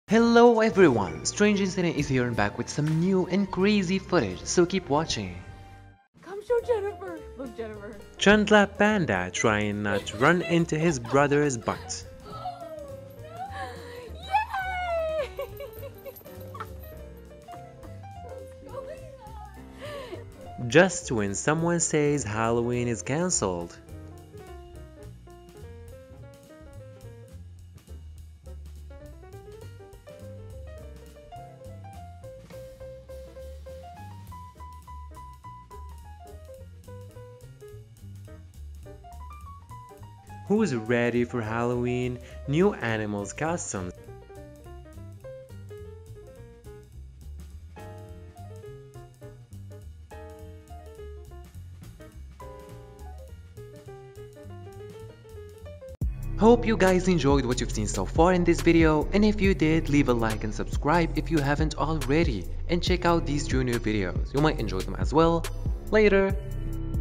Adorable Dogs Dressed In Panda Costumes
This is a short compilation video of 3 clips of 2 cute and funny puppies. First clip is where the two them wearing panda suits and running towards mom's office making some cute walkies and ready for Halloween, they make such a cute couple. The second one is pretty much the same but this time it's a great reaction to someone who says Halloween is cancelled. 3rd one is holloween costumes but different ones and that is of a lion, they again are the Cutest.

For more of these adorable little two panda halloween dogs, you can check them out on Instagram